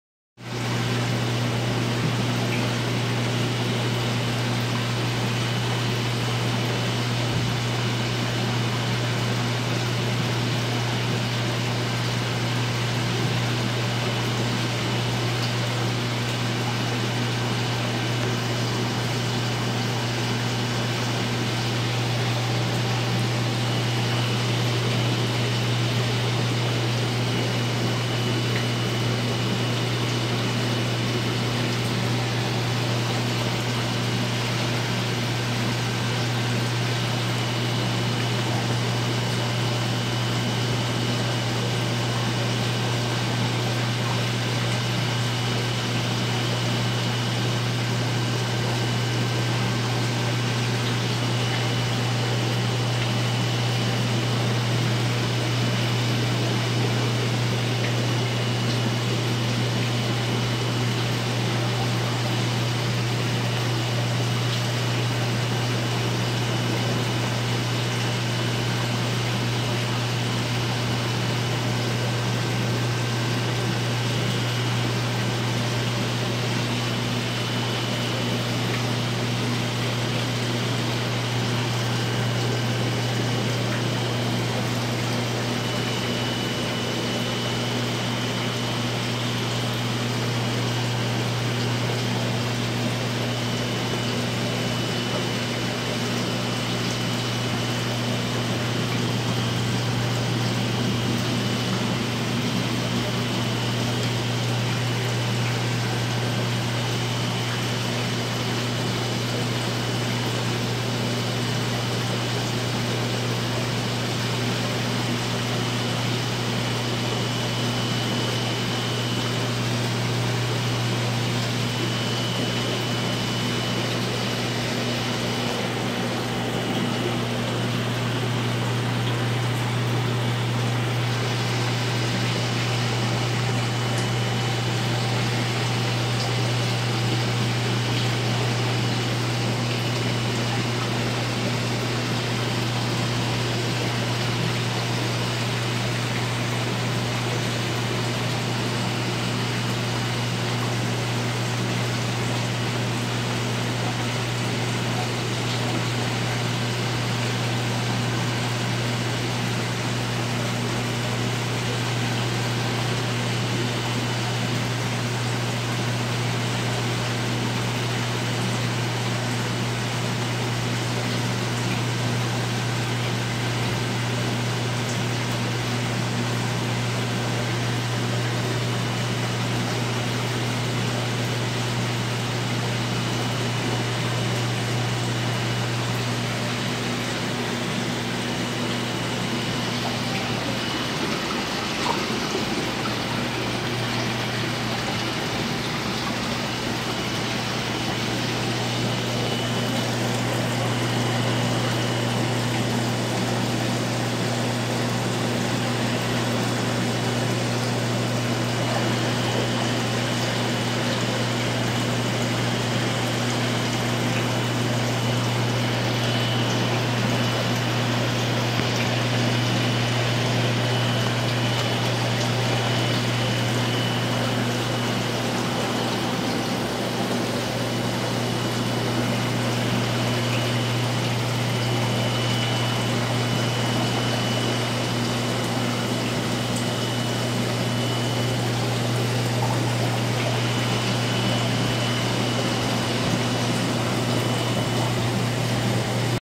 Yellowfin Tang $699 - Customer's Tank
approx 14 in and aggressively eating

Ed
the Reef Corner